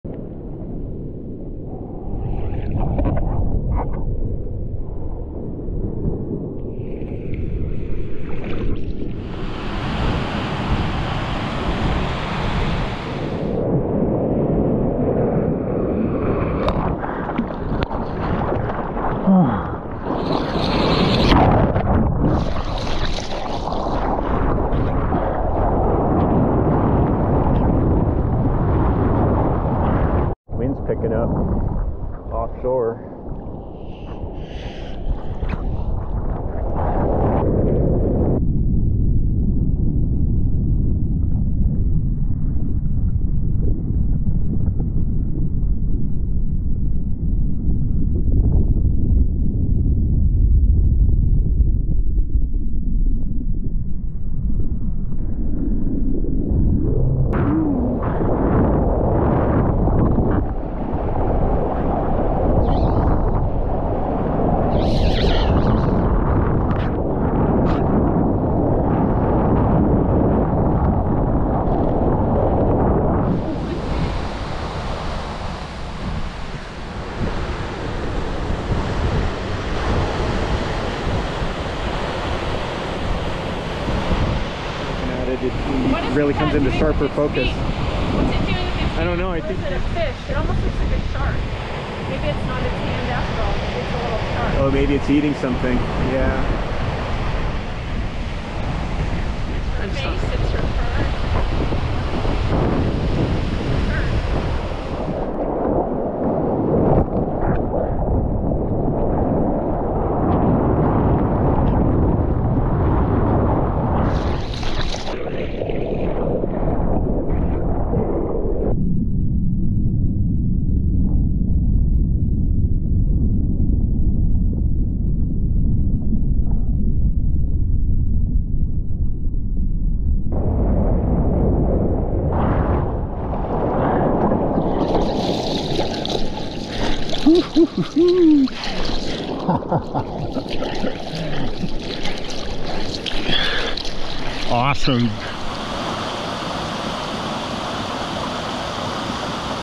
Low Tide Drainers At Graffiti Beach
Surfing some fun low tide drainers today. Clean and light offshore conditions, and a combo swell light up the arroyo with plenty of zippy peaks. It ain't Snapper Rock, but I'll take the score and run with it as far as I can go. The hidden stash of graffiti is pretty cool too. I'll show you a bit of it today. More of it in the episodes to follow. See you all again in a few days!